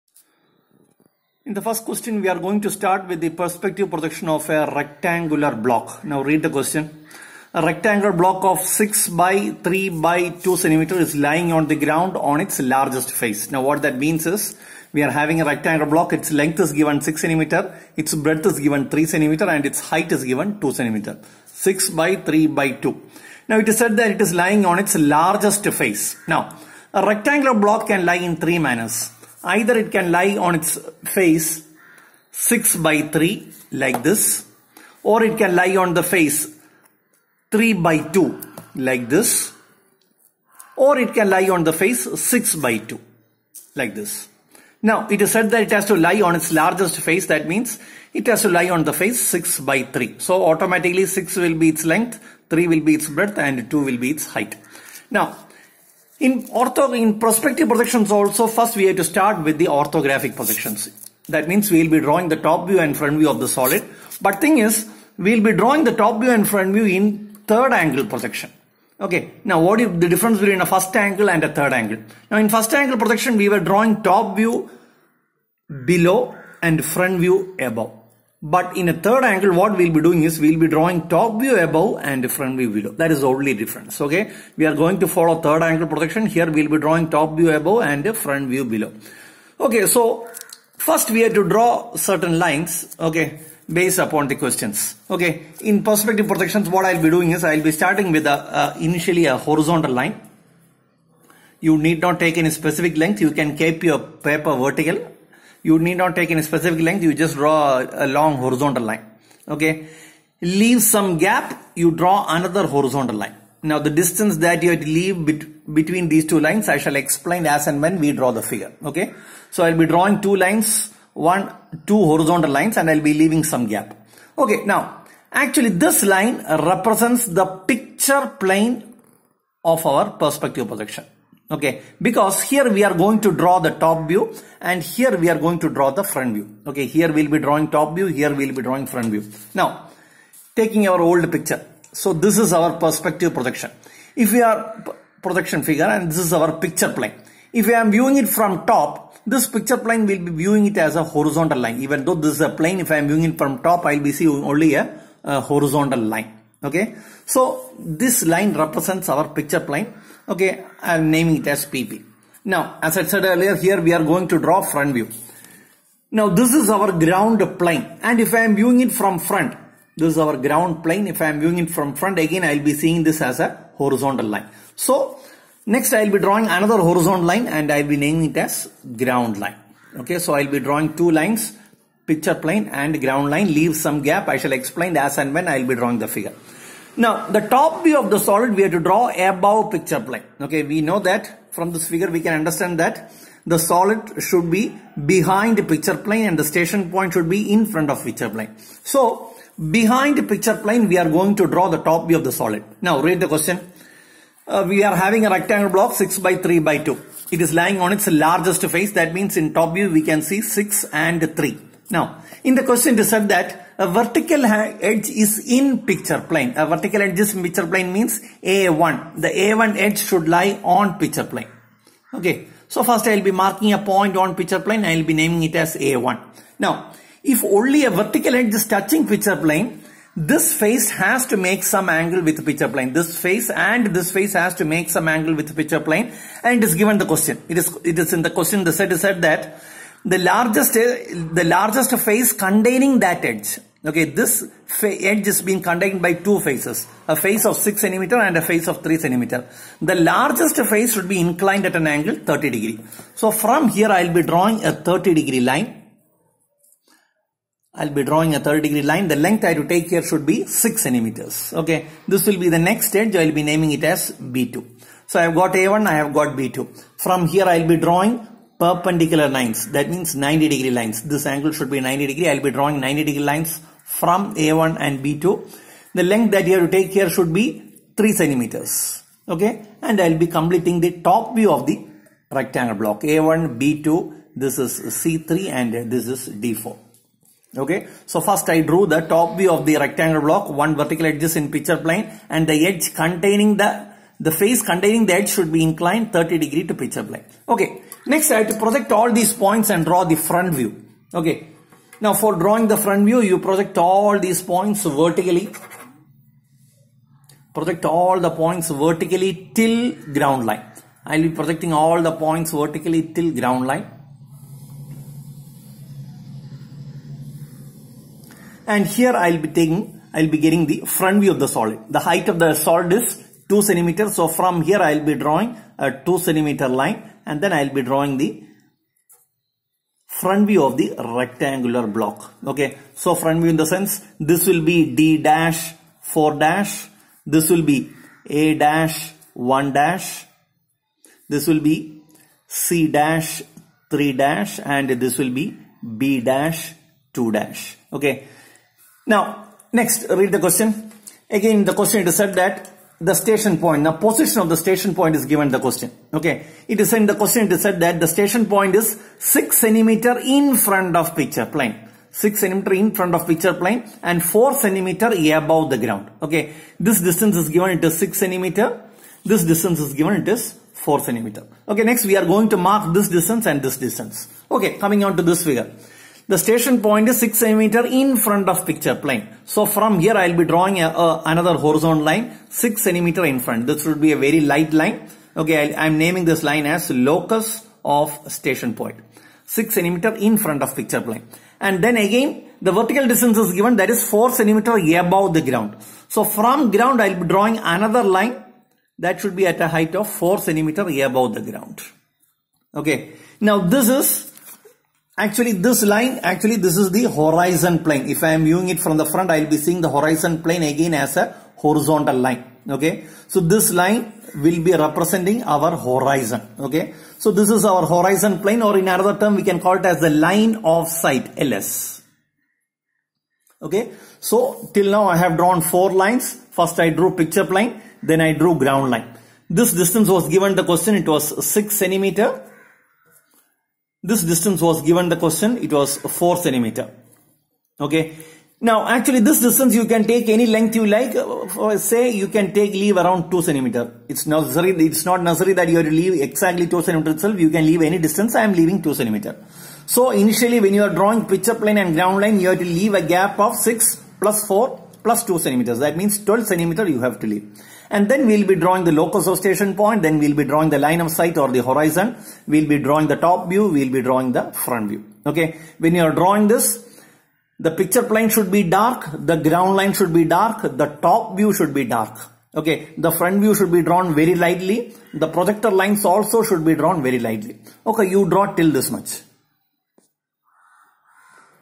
1a. Perspective Projection of a rectangular block
1a. A  rectangular  prism  of  6×3×2cm  is  lying  on  the  ground  with  one  of  its  largest faces.  A  vertical  edge  is  in  the  PP  and  the  large  face  containing  that  edge  makes an angle of 30º  with  PP. The SP is 6cm in  front of the  PP, 4cm  above the ground and lies in the central plane, which passes through the center of the block